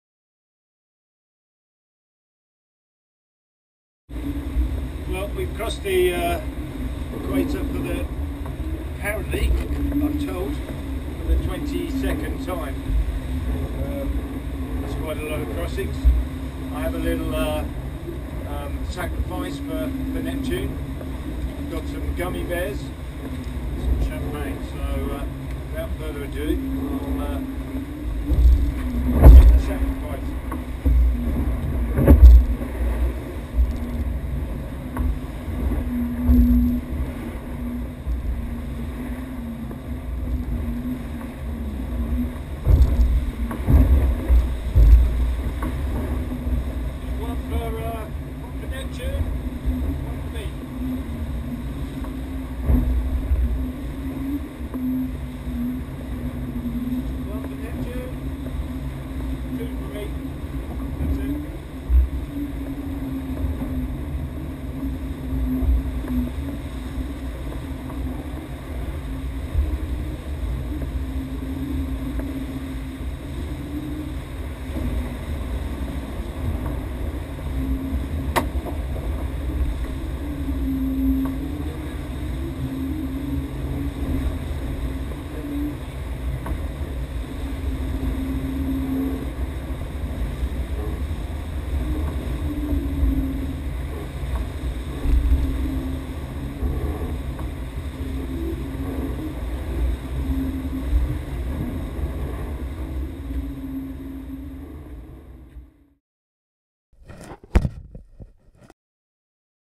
A Gift To Neptune - Mike Golding & Gamesa 22 Nov 2012 Vendée Globe
Mike Golding offers a sacrifice to the Roman God of the Water and Sea as he crosses the Equator for the 22nd time